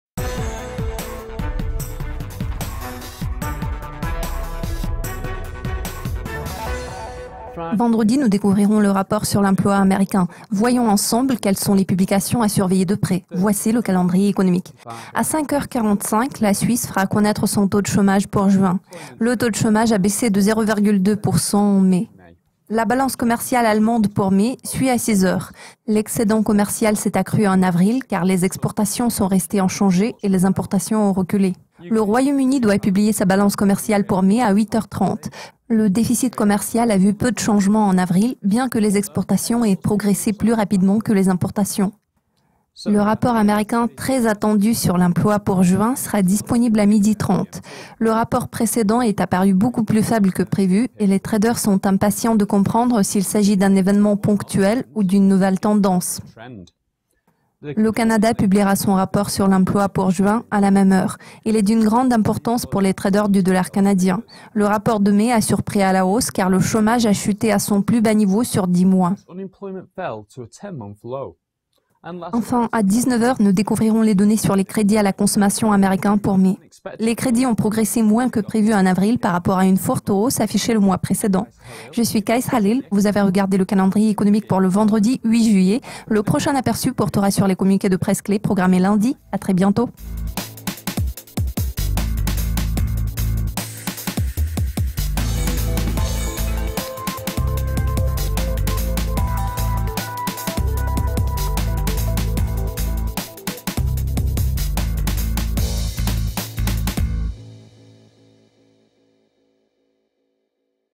Calendrier économique
Voici la liste des publications à surveiller le vendredi, 8 juillet.